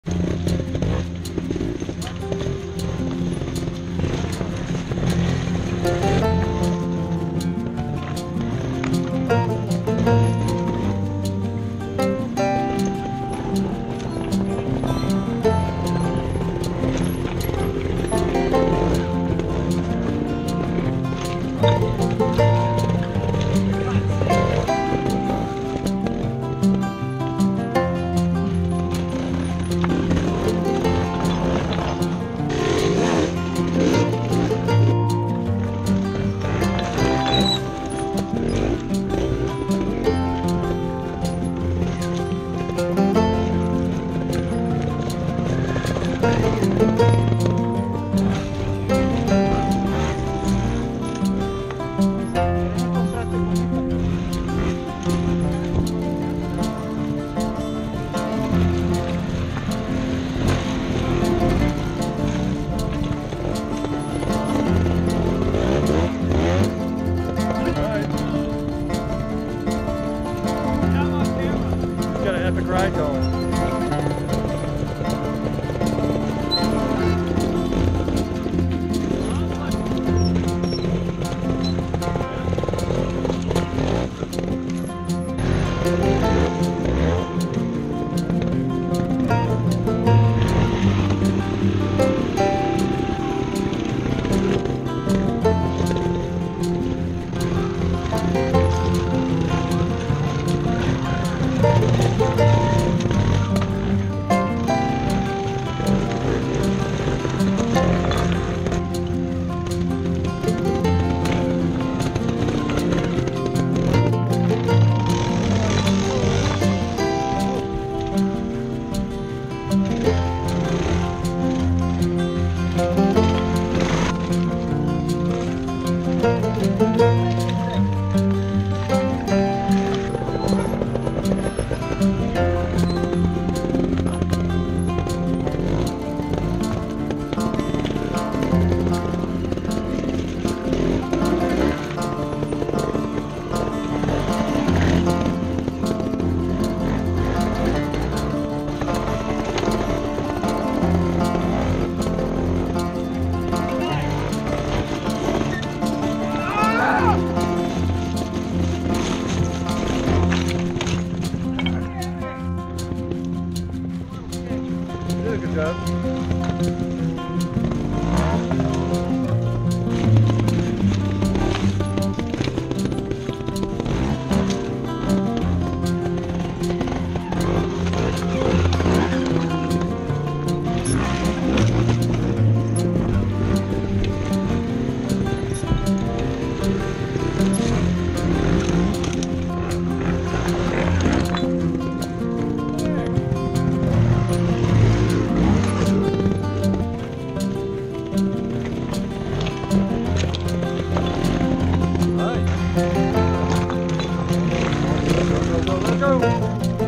CTTA GSL Ranch Trials Sat. PM 2019
Sections 7 & 8 of the GSL ranch trials in Kendalia TX.  7 was the toughest section of the day.